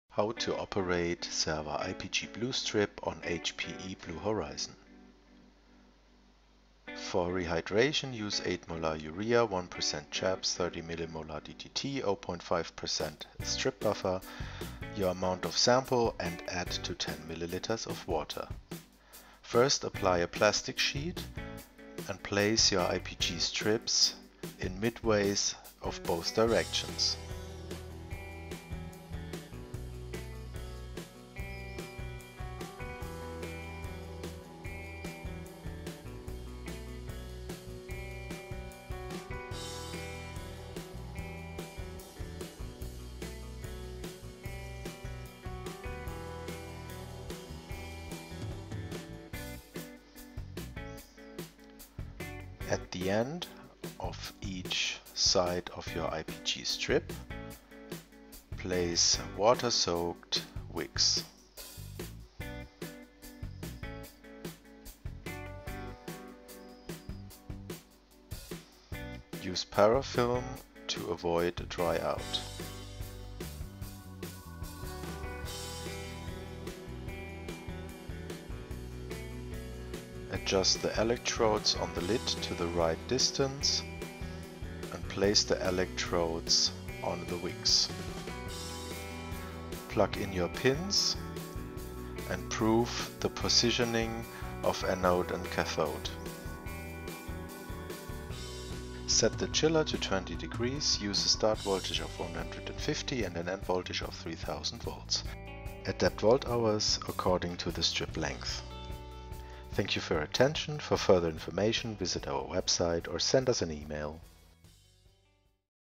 How to... operate IPG Strips using HPE BlueHorizon
Perform 1st dimension of 2D Electrophoresis on the HPE BlueHorizon.